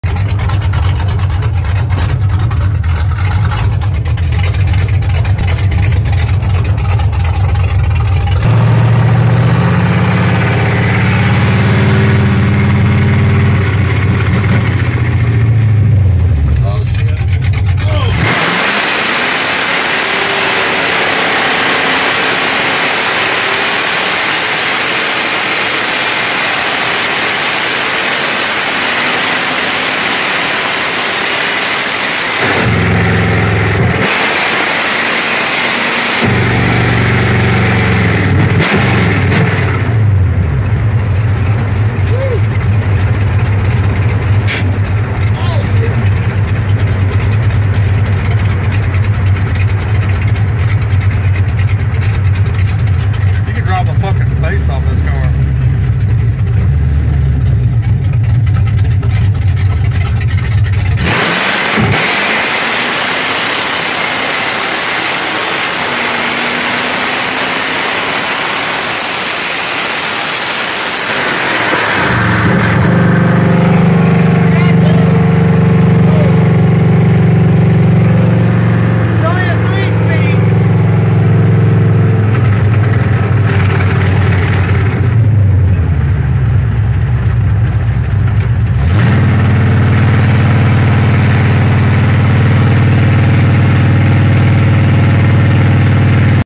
68 chevy chevelle. Open headers. Burnout.
Loud and powerful chevelle. Burnout on one lane road.